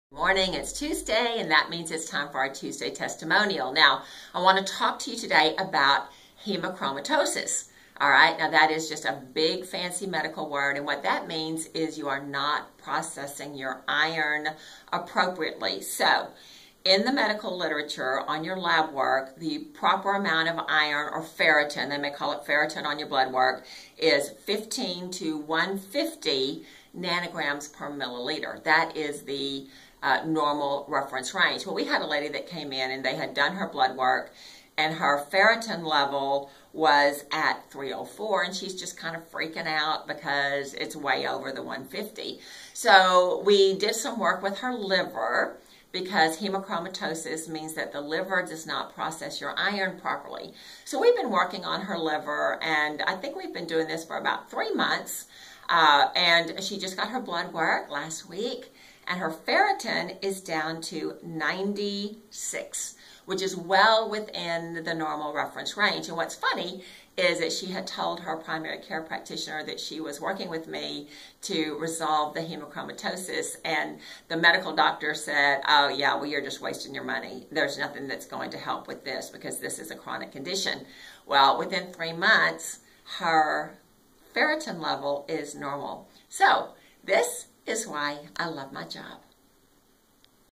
Hemochromatosis. Too much iron in the liver
Hedmochromatosis is a medical condition where the liver stores too much iron.  Listen how one person resolved this issue.